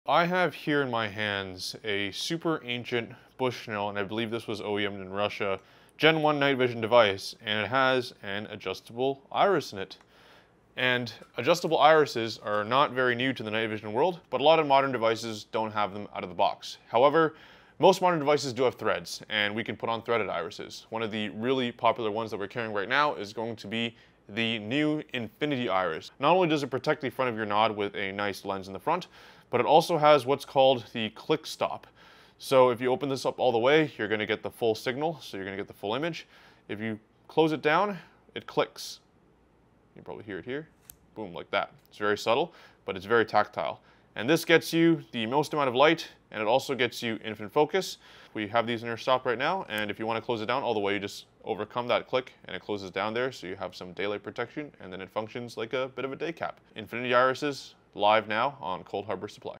Apertures and NODs: The Infinity Iris
The @i.infinityiris is a great attachment to any NOD allowing the user another aspect of control to their device. Apertures play around with restricting the amount of light in a device by way of closing an opening, not only changing the brightness of your image, but changing the depth of field.

Unique from other iris devices on the market is Click-Stop™ technology, which affords the user tactile feedback when the aperture is set to infinity.